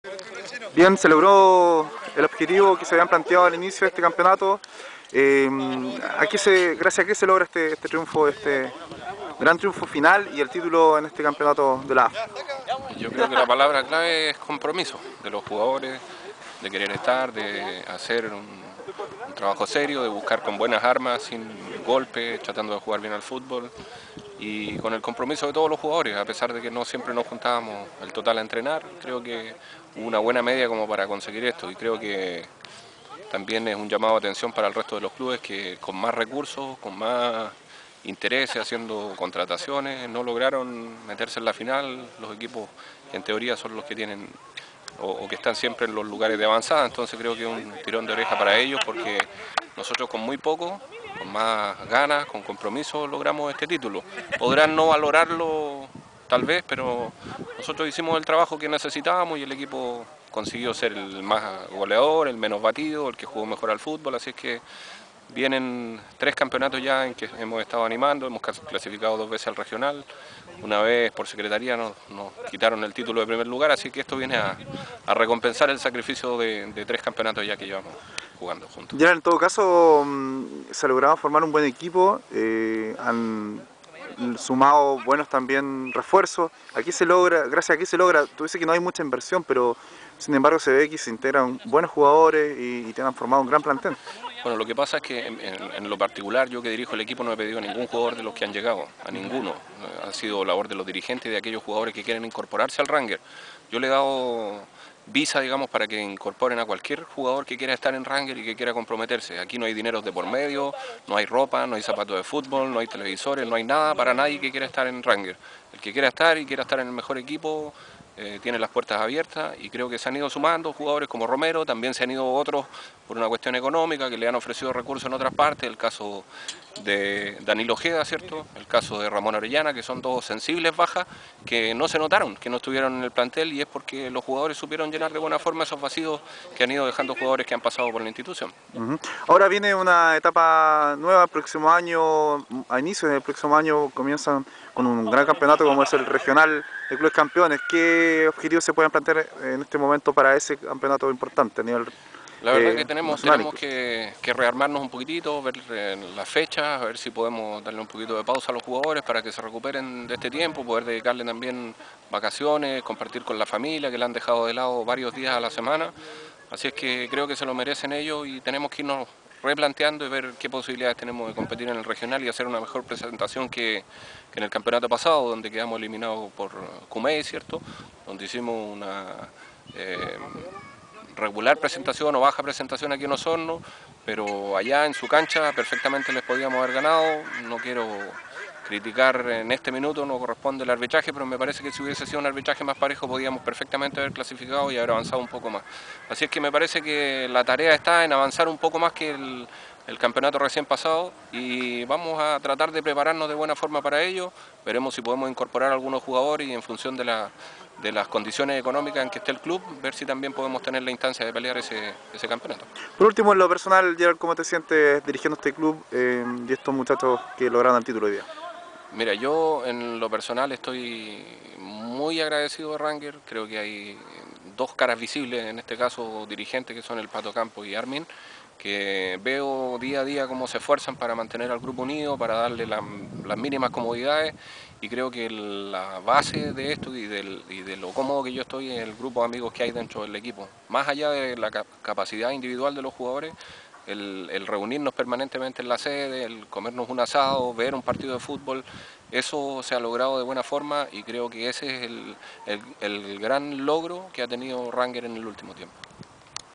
Gerhard Reiher, DT de Rangers, tras título del Clausura 2010
Rangers celebró con todo su título del Clausura 2010, luego de superar por 4 a 0 a Bancario.